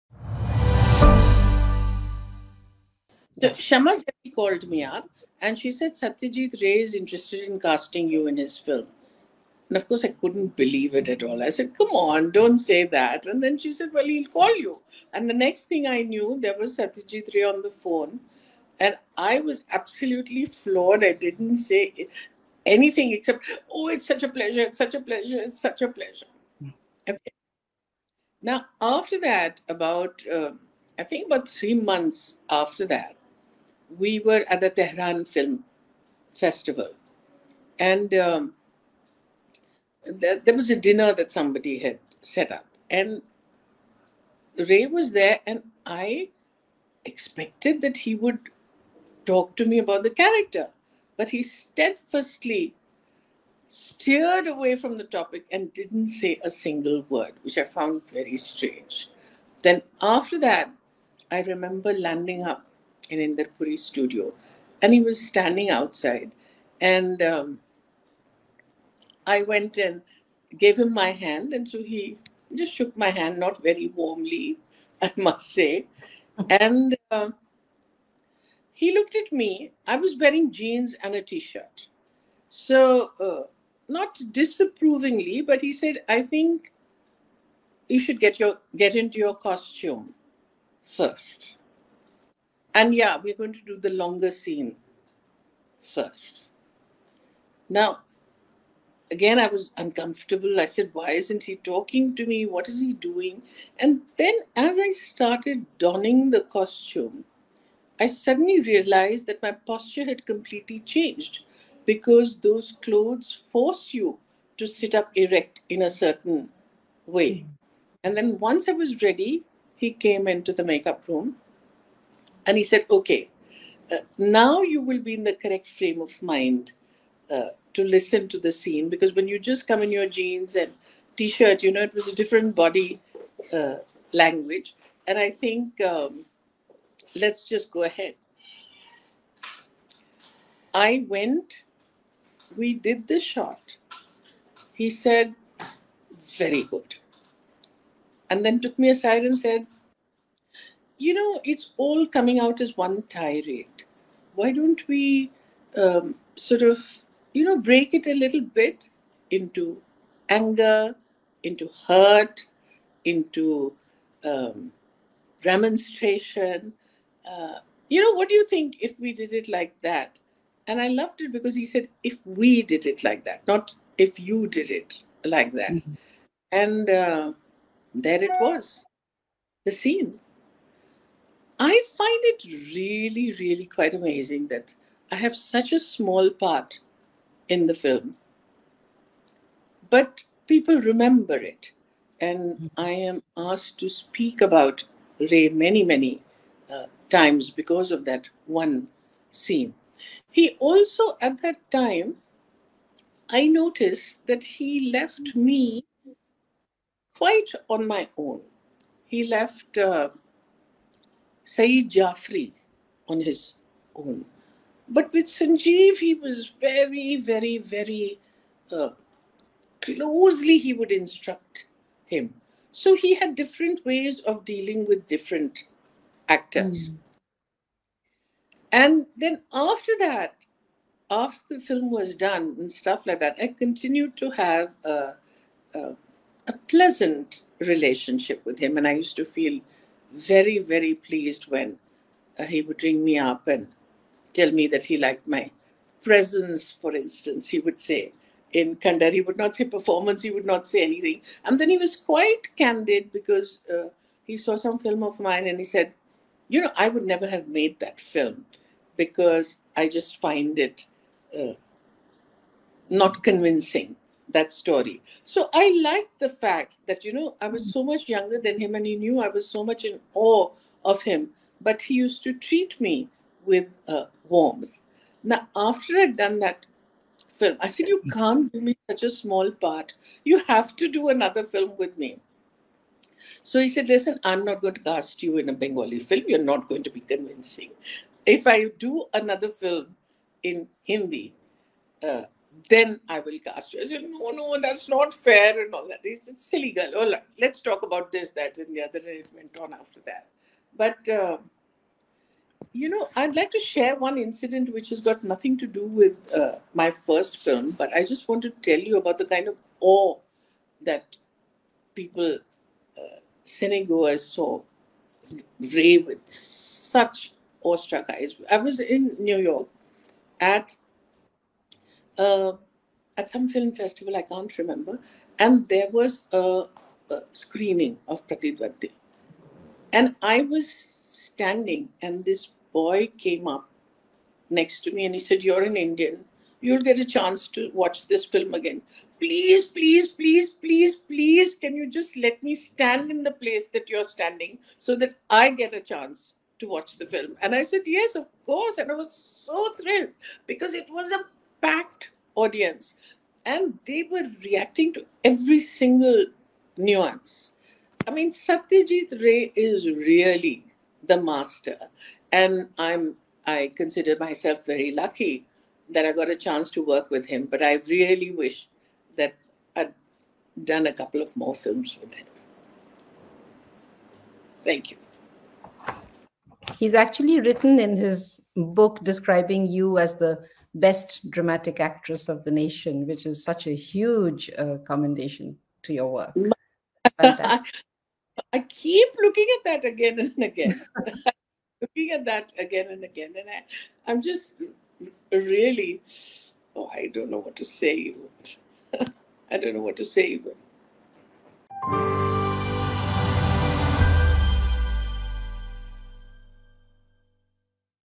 Shabana Azmi on working with Satyajit Ray
The award-winning actor relives being directed by the legendary Indian filmmaker in conversation with filmmaker-singer Sangeeta Datta for a Baithak UK special in the lead up to this year’s London Indian Film Festival (LIFF).